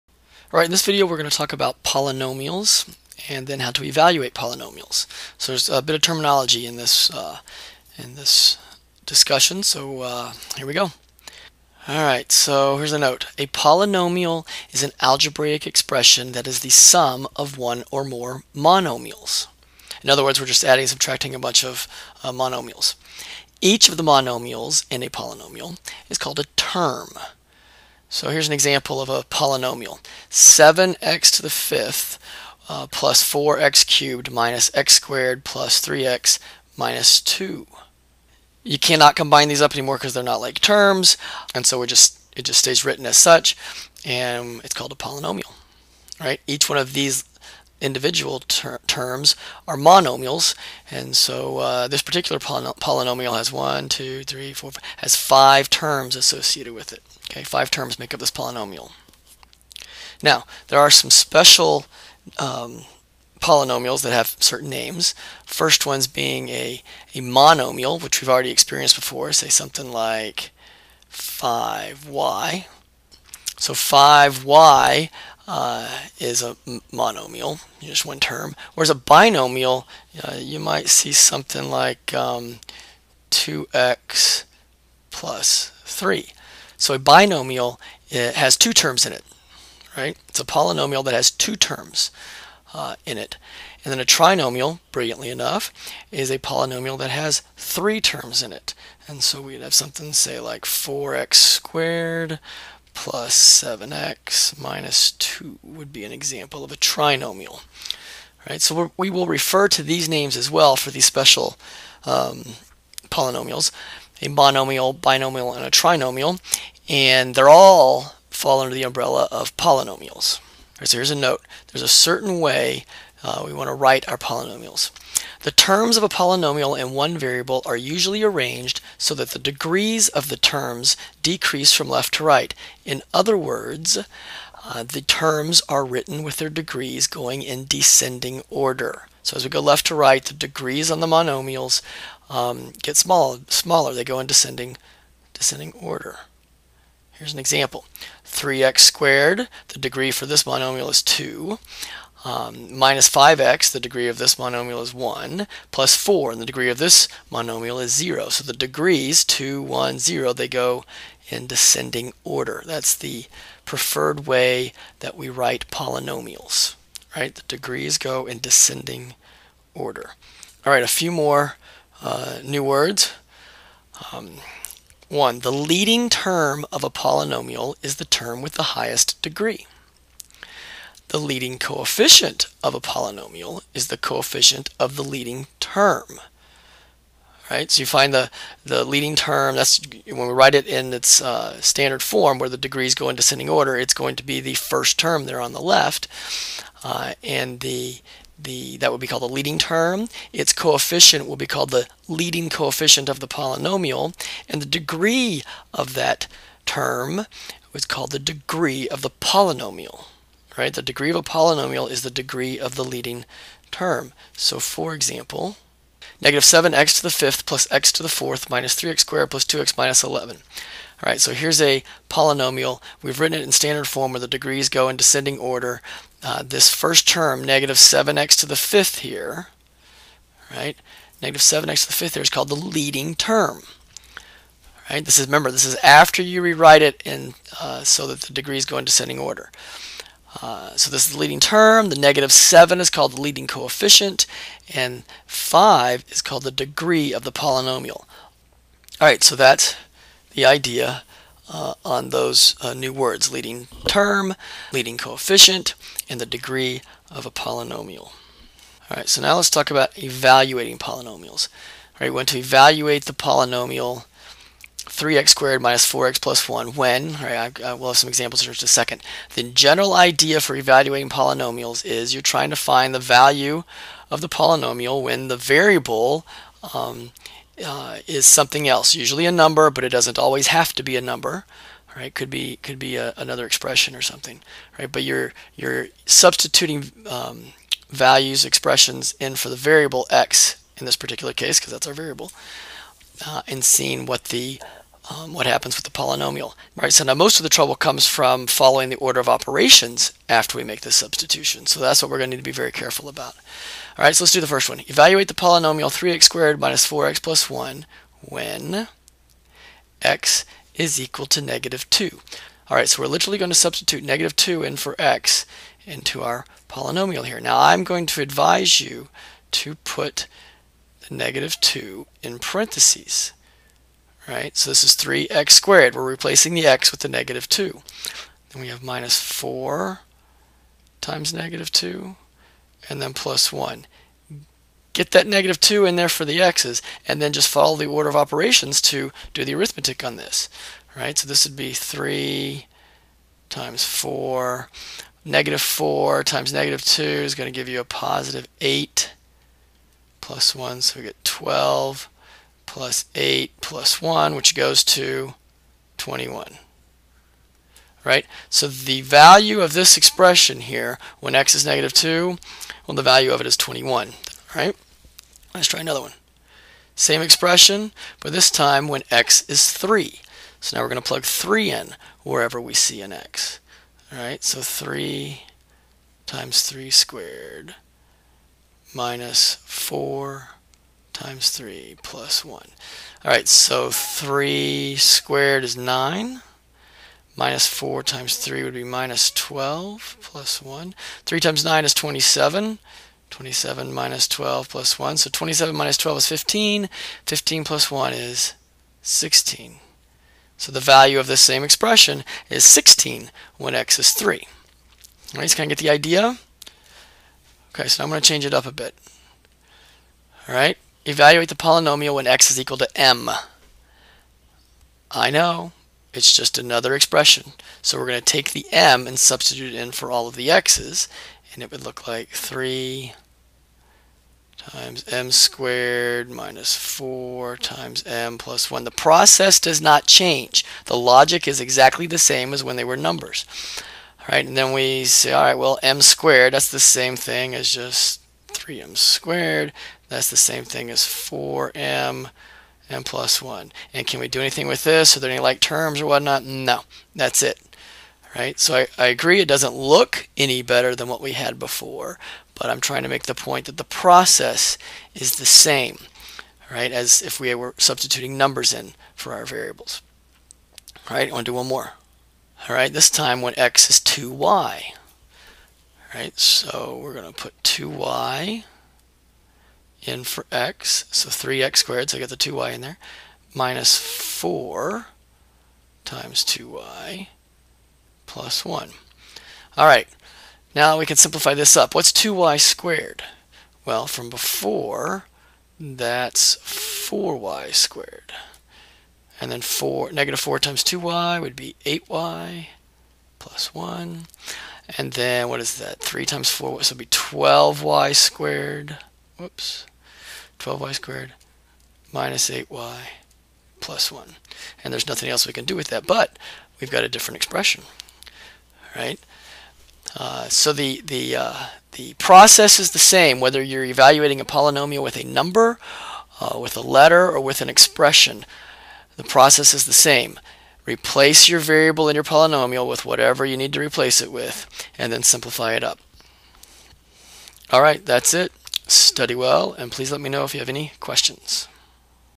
Polynomials
An introduction to polymomials followed by examples of evaluating polynomials.